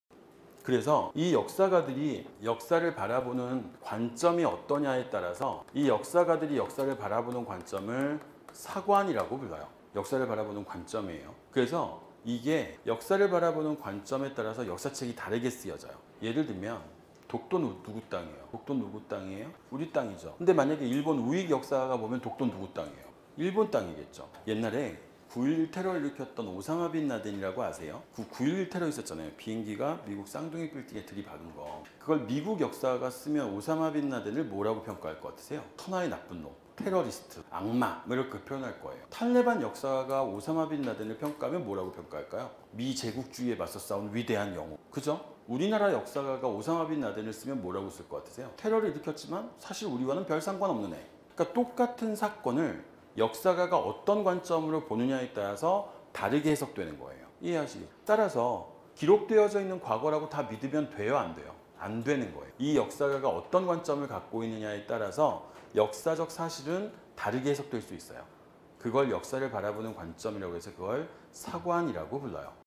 한국사를 배운다면 사관이란? 독도를 예를들어 알려줄게! '★독도는 한국땅★' l한국사 임찬호l한국사lKF소방사관l
역사가가 바라보는 역사의 관점에따라, 역사는 달라진다!
그렇다면 '사관'이라는것은 어떤의미일까?
독도를 예를들어 아주 이해하기쉽게 알려줄게!!
지금 바로 PLAY ▶ 티치온 "임찬호 한국사'